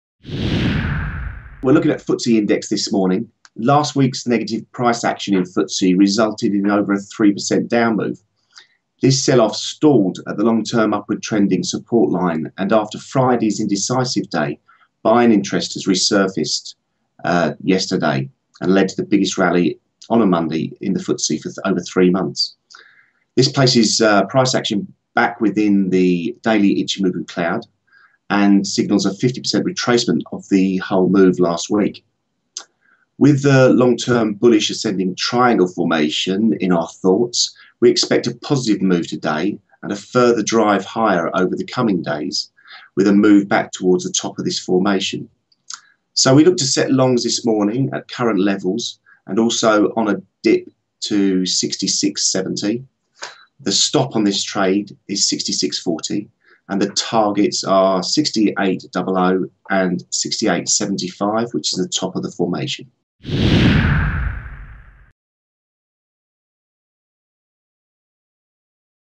O'Hare: Looking for FTSE lift off
Steve O'Hare from First4Trading is looking for more positive moves in
the FTSE 100 index this Tuesday. He believes last week's sell-off has
stalled at the long-term upward trending support line and there could
be more gains today. He says price action is back within the daily
Ichimoku cloud cover and signals a 50% retracement of last week's move
lower.

Steve believes that with this long-term bullish ascending triangle
formation still in place,  a further drive higher is possible with a
move back towards the top of this formation over the coming days. He
aims to set longs from the open and on a pull-back lower towards the
supporting trendline.

The UK will be in focus today; The Bank of England Governor, Mark
Carney is addressing policymakers. We also get UK inflation data,
although this is unlikely to provide too many surprises.

Stop*:* 6640